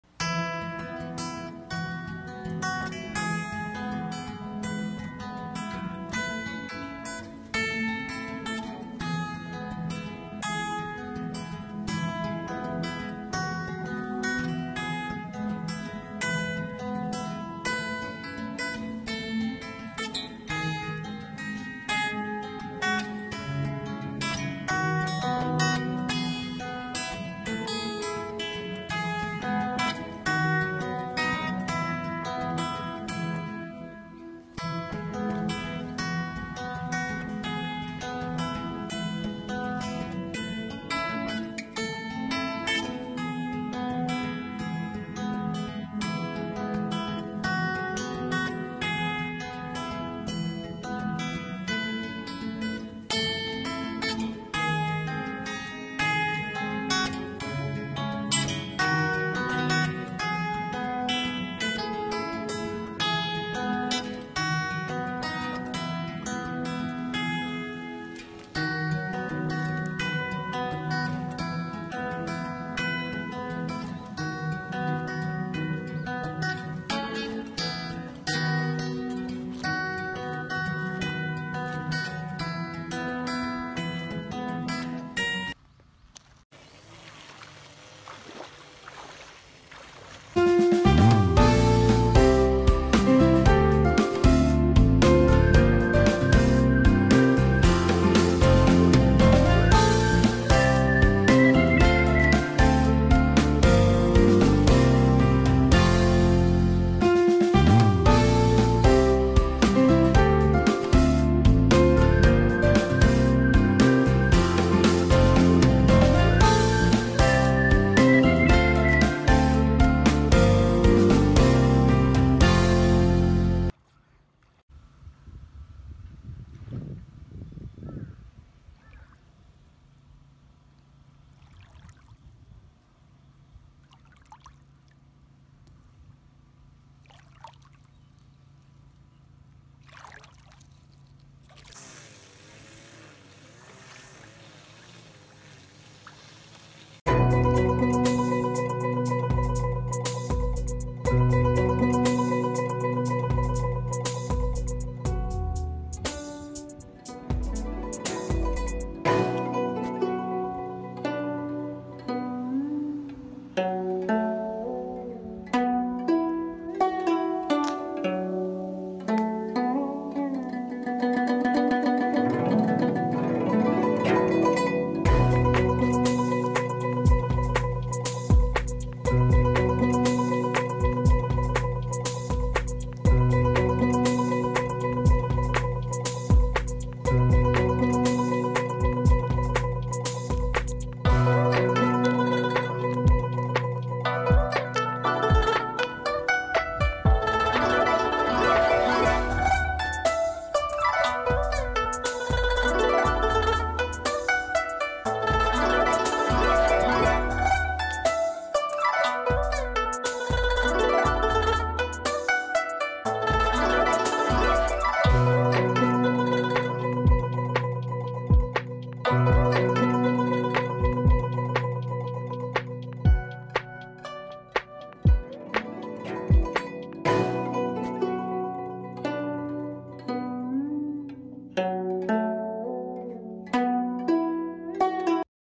🐠 conociendo un río de Japón 🐠
Hola
Hoy día quise compartir mi paseó a un rio de Japón donde podrás admirar hermosos paisajes y mucha naturaleza
Gracias